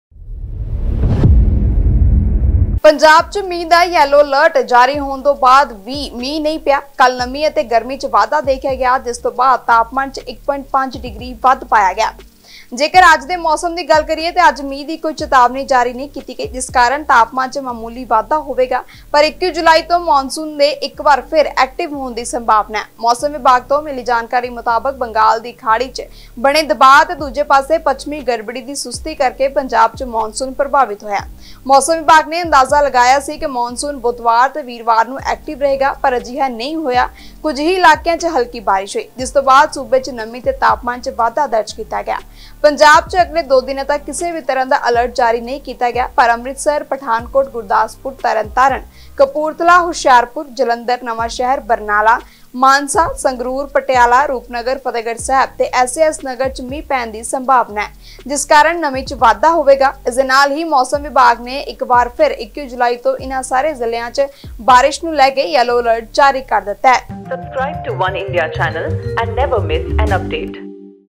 ਮੌਸਮ ਨੇ ਬਦਲਿਆ ਮਿਜ਼ਾਜ, ਜਾਣੋ ਕਦੋਂ ਪਵੇਗਾ ਮੀਂਹ | Oniendia Punjabi
ਪੰਜਾਬ 'ਚ ਪਵੇਗਾ ਭਾਰੀ ਮੀਂਹ! ਹੋ ਜਾਓ ਸਾਵਧਾਨ!
ਇਨ੍ਹਾਂ ਜ਼ਿਲ੍ਹਿਆਂ 'ਚ Alert ਕਰ'ਤਾ ਜਾਰੀ

ਪੰਜਾਬ 'ਚ ਮੀਂਹ ਦਾ ਯੈਲੋ ਅਲਰਟ ਜਾਰੀ ਹੋਣ ਤੋਂ ਬਾਅਦ ਵੀ ਮੀਂਹ ਨਹੀਂ ਪਿਆ।  ਕੱਲ੍ਹ ਨਮੀ ਅਤੇ ਗਰਮੀ 'ਚ ਵਾਧਾ ਦੇਖਿਆ ਗਿਆ, ਜਿਸ ਤੋਂ ਬਾਅਦ  ਤਾਪਮਾਨ 'ਚ 1.5 ਡਿਗਰੀ ਵੱਧ ਪਾਇਆ ਗਿਆ। ਜੇਕਰ ਅੱਜ ਦੇ ਮੌਸਮ ਦੀ ਗੱਲ ਕਰੀਏ ਤਾਂ ਅੱਜ ਮੀਂਹ ਦੀ ਕੋਈ ਚਿਤਾਵਨੀ ਜਾਰੀ ਨਹੀਂ ਕੀਤੀ ਗਈ। ਜਿਸ ਕਾਰਨ ਤਾਪਮਾਨ 'ਚ ਮਾਮੂਲੀ ਵਾਧਾ ਹੋਵੇਗਾ। ਪਰ 21 ਜੁਲਾਈ ਤੋਂ ਮਾਨਸੂਨ ਦੇ ਇੱਕ ਵਾਰ ਫਿਰ ਐਕਟਿਵ ਹੋਣ ਦੀ ਸੰਭਾਵਨਾ ਹੈ।  ਮੌਸਮ ਵਿਭਾਗ ਤੋਂ ਮਿਲੀ ਜਾਣਕਾਰੀ ਅਨੁਸਾਰ ਬੰਗਾਲ ਦੀ ਖਾੜੀ ਵਿੱਚ ਬਣੇ ਦਬਾਅ ਅਤੇ ਦੂਜੇ ਪਾਸੇ ਪੱਛਮੀ ਗੜਬੜੀ ਦੀ ਸੁਸਤੀ ਕਰਕੇ ਪੰਜਾਬ ਵਿੱਚ ਮਾਨਸੂਨ ਪ੍ਰਭਾਵਿਤ ਹੋਇਆ ਹੈ। ਮੌਸਮ ਵਿਭਾਗ ਨੇ ਅੰਦਾਜ਼ਾ ਲਗਾਇਆ ਸੀ ਕਿ ਮਾਨਸੂਨ ਬੁੱਧਵਾਰ ਅਤੇ ਵੀਰਵਾਰ ਨੂੰ ਐਕਟਿਵ ਰਹੇਗਾ ਪਰ ਅਜਿਹਾ ਨਹੀਂ ਹੋਇਆ। ਕੁਝ ਹੀ ਇਲਾਕਿਆਂ 'ਚ ਹਲਕੀ ਬਾਰਿਸ਼ ਹੋਈ। ਜਿਸ ਤੋਂ ਬਾਅਦ ਸੂਬੇ 'ਚ ਨਮੀ ਅਤੇ ਤਾਪਮਾਨ 'ਚ ਵਾਧਾ ਦਰਜ ਕੀਤਾ ਗਿਆ ਹੈ।  ਪੰਜਾਬ 'ਚ ਅਗਲੇ ਦੋ ਦਿਨਾਂ ਤੱਕ ਕਿਸੇ ਵੀ ਤਰ੍ਹਾਂ ਦਾ ਅਲਰਟ ਜਾਰੀ ਨਹੀਂ ਕੀਤਾ ਗਿਆ ਹੈ। ਪਰ ਪਠਾਨਕੋਟ, ਗੁਰਦਾਸਪੁਰ, ਅੰਮ੍ਰਿਤਸਰ, ਤਰਨਤਾਰਨ, ਕਪੂਰਥਲਾ, ਹੁਸ਼ਿਆਰਪੁਰ, ਜਲੰਧਰ, ਨਵਾਂਸ਼ਹਿਰ, ਬਰਨਾਲਾ, ਮਾਨਸਾ, ਸੰਗਰੂਰ, ਪਟਿਆਲਾ, ਰੂਪਨਗਰ, ਫਤਿਹਗੜ੍ਹ ਸਾਹਿਬ ਤੇ ਐਸਏਐਸ ਨਗਰ 'ਚ ਮੀਂਹ ਪੈਣ ਦੀ ਸੰਭਾਵਨਾ ਹੈ। ਜਿਸ ਕਾਰਨ ਨਮੀ ਵਧੇਗੀ। ਇਸ ਦੇ ਨਾਲ ਹੀ ਮੌਸਮ ਵਿਭਾਗ ਨੇ ਇਕ ਵਾਰ ਫਿਰ 21 ਜੁਲਾਈ ਤੋਂ ਇਨ੍ਹਾਂ ਸਾਰੇ ਜ਼ਿਲ੍ਹਿਆਂ 'ਚ ਬਾਰਿਸ਼ ਨੂੰ ਲੈ ਕੇ ਯੈਲੋ ਅਲਰਟ ਜਾਰੀ ਕੀਤਾ ਗਿਆ ਹੈ।